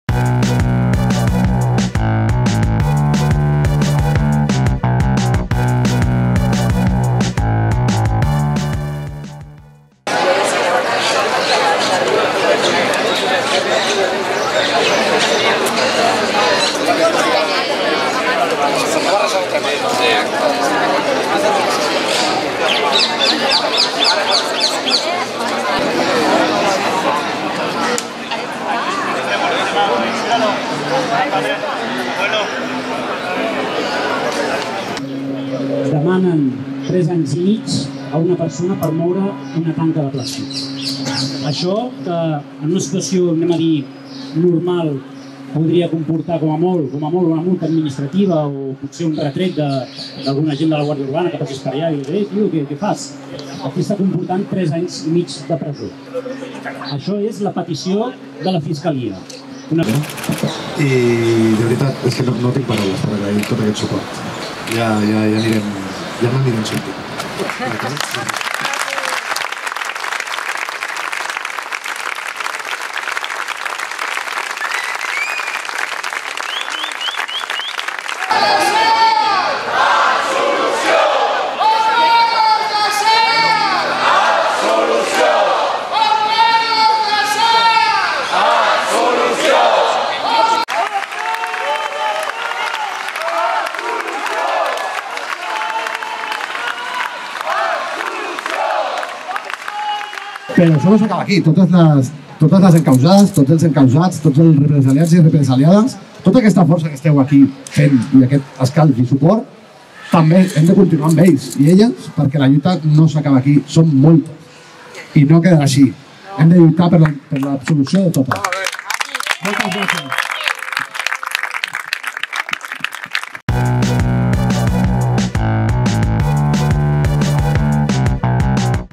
Suport en el judici a en "Carles de Sants" [ contrainfos ]
Quasi un centenar de persones donen suport a en Carles de Sants en el judici en el qual li demanen 3 anys i mig de presó. El jove va ser detingut durant les protestes per l'empresonament del raper Pablo Hasel.